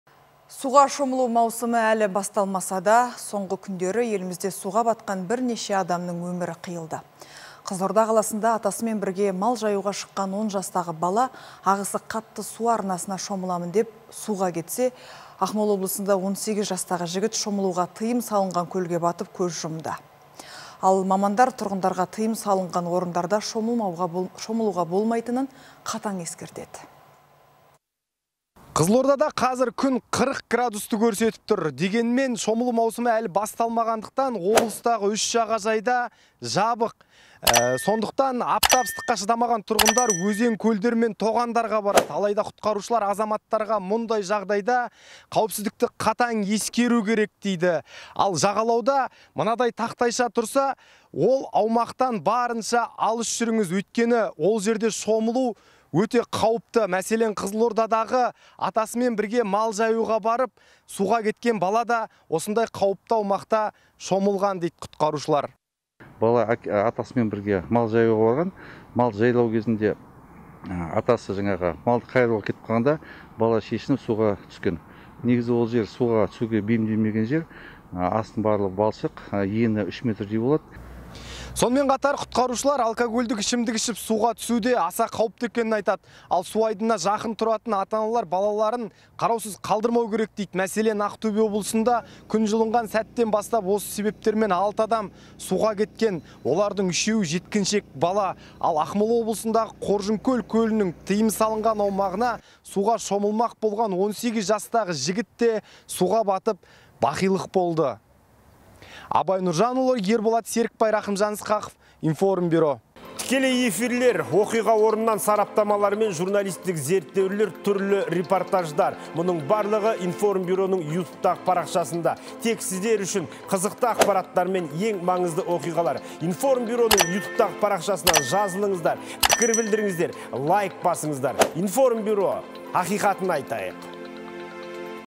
Шомылу маусымы басталмай жатып ажалы судан болған бірнеше адам тіркелді
Суға шомылу маусымы әлі басталмаса да, соңғы күндері елімізде суға батқан бірнеше адамның өмірі қиылды. Қызылорда қаласында атасымен бірге мал жаюға шыққан он жастағы бала ағысы қатты су арнасына шомыламын деп, суға кетсе,  Ақмола облысында 18 жастағы жігіт шомылуға тыйым салынған көлге батып көз жұмды. Ал мамандар тұрғындарға тыйым салынған орындарда шомылмауға болмайтынан қатаң ескертеді.
АБАЙ НҰРЖАНҰЛЫ, тілші:
– Қазір Қызылордада күн 40 градусты көрсетіп тұр. Дегенмен, шомылу маусымы басталмағандықтан облыстағы суға түсуге болатын үш жағажай да жабық. Сондықтан аптап ыстыққа шыдамаған тұрғындар тоғандар мен өзен-көлдерге барады. Бірақ құтқарушылар азаматтарға мұндай жағдайда қауіпсіздікті қатаң ескеру керек дейді. Ал жағалауда мынадай тақтайша тұрса, ол аумақтан барынша алыс жүріңіз. Өйткені ол жерге шомылу - қауіпті. Мәселен, Қызылордадағы атасымен бірге мал жаюға барып, суға кеткен бала да сондай қауіпті жерге шомылған.
БАХЫТ ЕМБЕРГЕНОВ, Қызылорда облысы ТЖД жедел құтқару жасағының басшысы:
– Біз барғанша атасы баласын шығарыпты. Баласы атасымен бірге мал жаюға барған. Атасы мал жаюға кетіп қалғанда, бала суға түскен. Негізі ол жер суға түсуге бейімделмеген жер. Асты балшық. Ені 3 метрдей, тереңдігі 1 метрдей болады. Бейімделмеген жерге суға түскесін суға батып кетіп отыр.
АБАЙ НҰРЖАНҰЛЫ, тілші:
– Сонымен қатар құтқарушылар алкогольдік ішімдік ішіп суға түсу де аса қауіпті екенін айтады. Ал су айдынына жақын тұратын ата-аналар балаларын қараусыз қалдырмау керек дейді. Мәселен Ақтөбе облысында күн жылынған сәттен бастап осы себептермен алты адам суға кеткен. Олардың 3-еуі жеткіншек бала. Ал Ақмола облысындағы Қоржынкөл көлінің тыйым салынған аумағына суға шомылмақ болған 18 жастағы жігіт те суға батып бақилық болды.

Абай Нұржанұлы, Ерболат Серікбай, Рахымжан Ысқақов